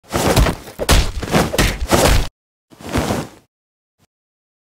The secret handshake
the secret handshake follows a story on which superior Spider-Man and Ben Reilly have met up and must do their secret handshake